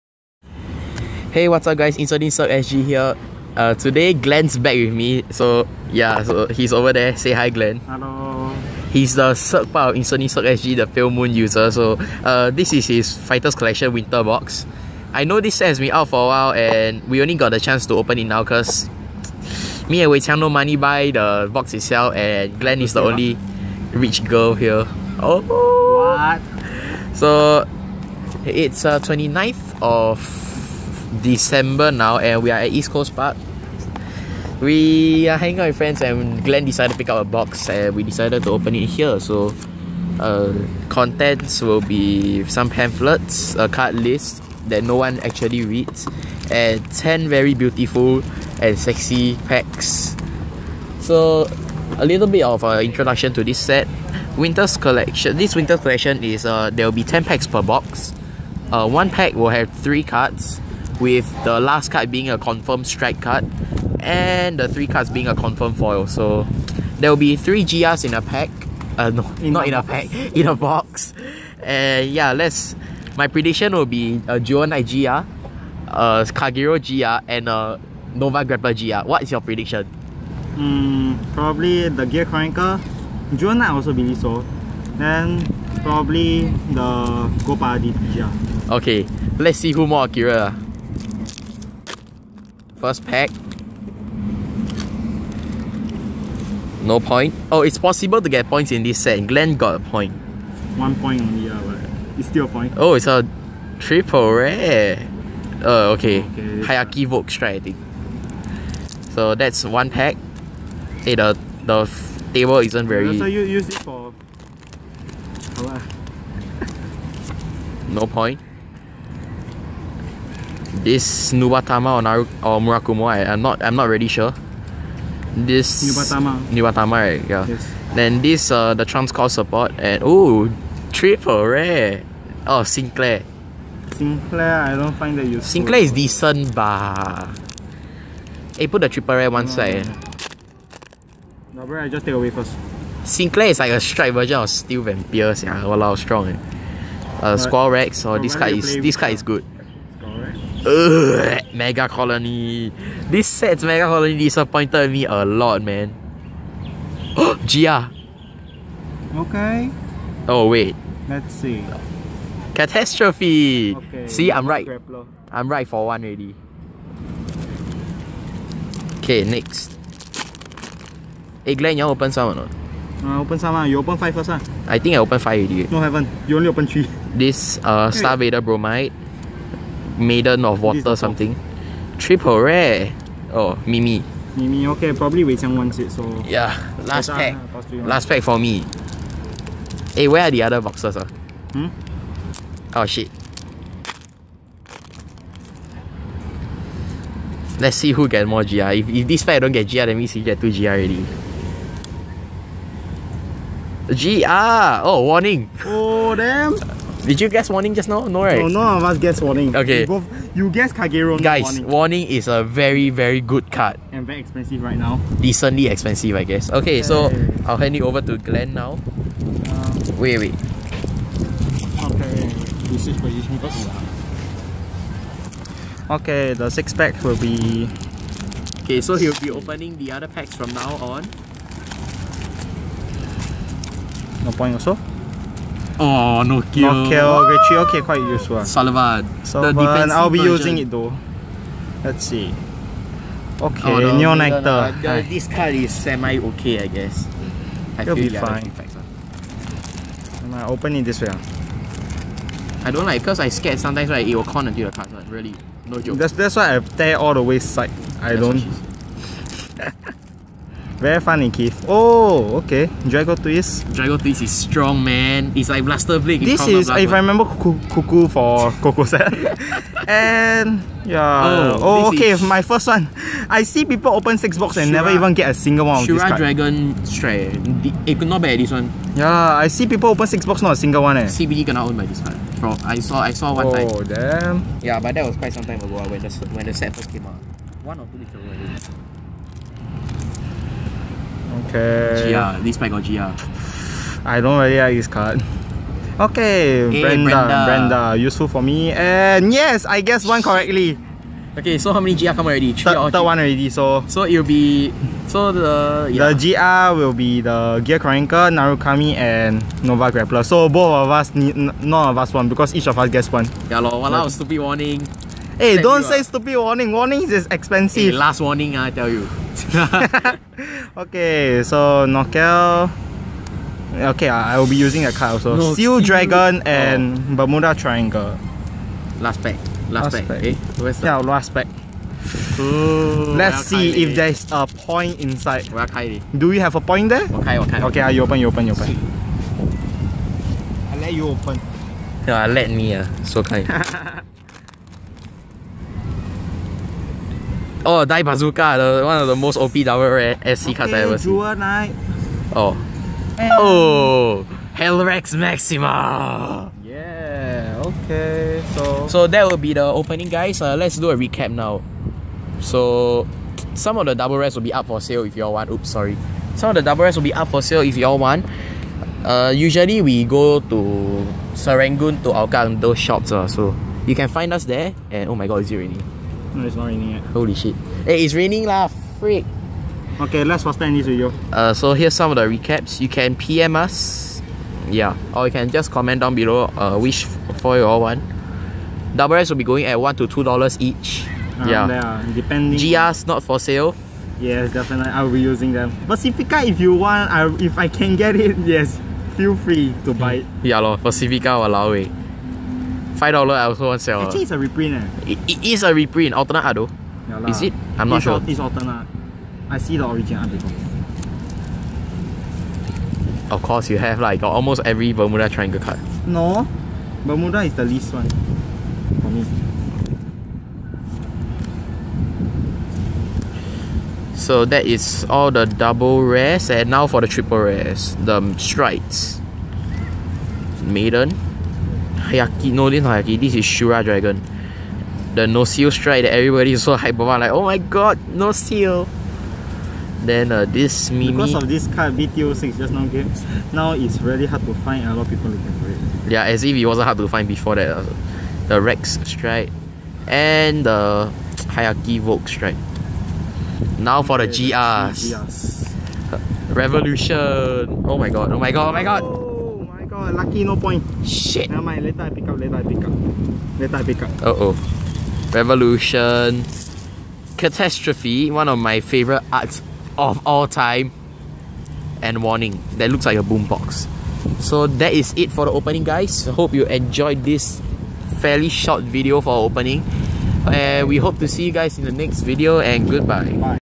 InseDinsCirqueSG's WinterFC2015 Box Opening
Hey Guys Keith from InseDinsCirqueSG here!
RRs will be going at $1-$2 per piece depending on Availability
P.O.C will only be at TTZ Hougang